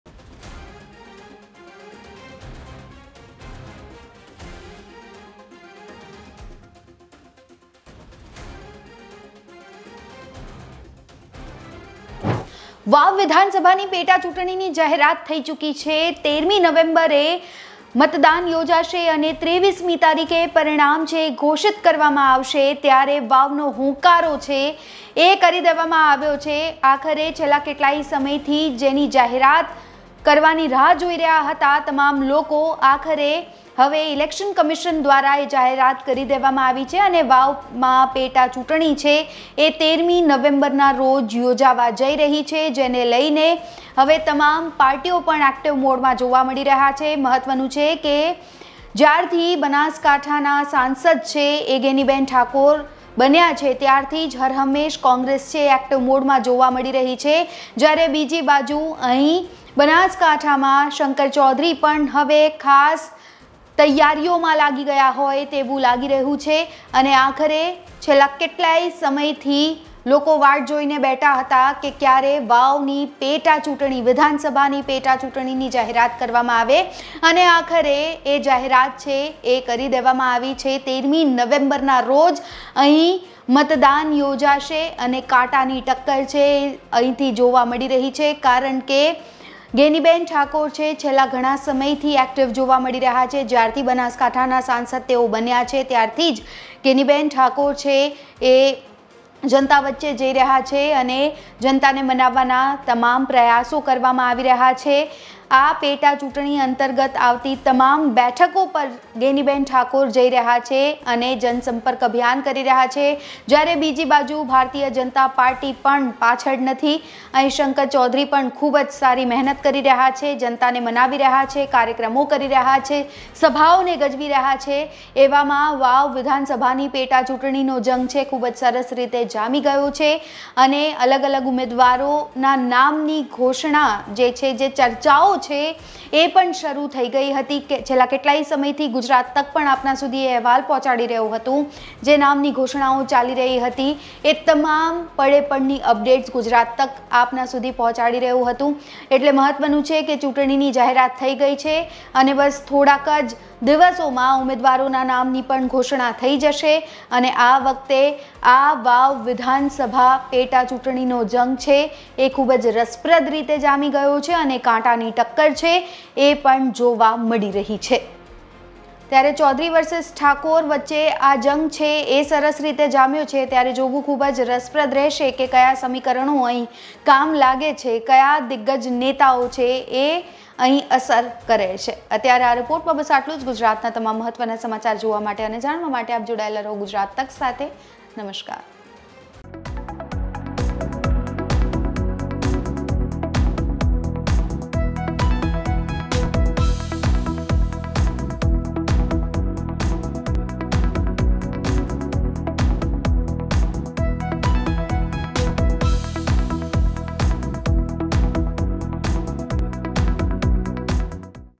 Vav By Elections : BJP Gujaratમાં જ્યાં હાર્યું એ વાવ બેઠક પર પેટાચૂંટણીની જાહેરાત!
Vav Assembley By Elections : ભાજપ ગુજરાતમાં જ્યાં હાર્યું એ વાવ બેઠક પર પેટાચૂંટણીની જાહેરાત, નવેમ્બરમાં આ તારીખે થશે